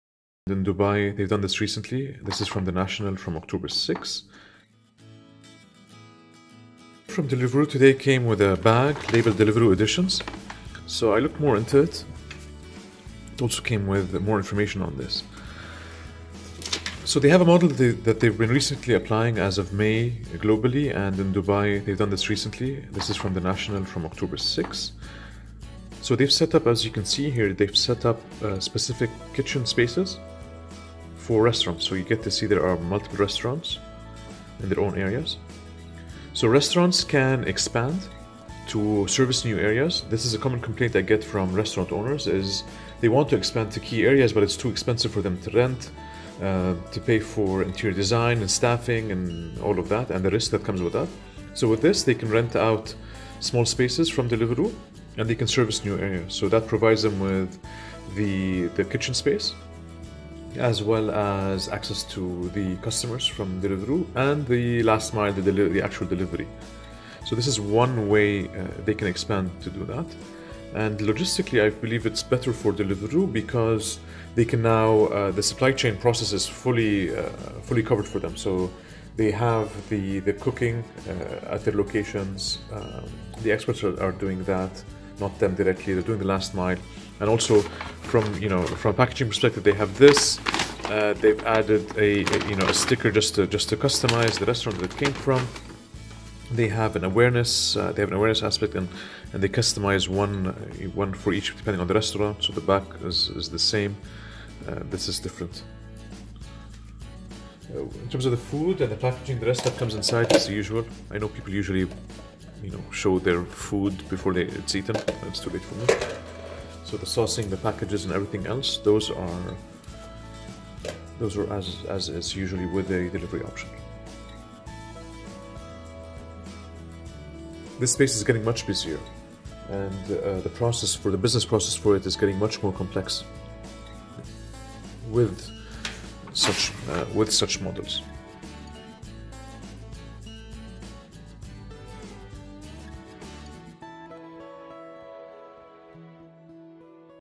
Deliveroo Editions - Dubai - Changing Restaurant's Business Process
Deliveroo Editions launches in Dubai in 2017. New kitchen setup that they launched in May 2017 globally and recently launched in Dubai to enable restaurants to expand to key areas cost effectively.  Moreover, Deliveroo benefits by having a more efficient logistical process and a tighter tie in with restaurants (vs. Uber Eats and others).   This changes the restaurant's business process. Check out how they're doing it.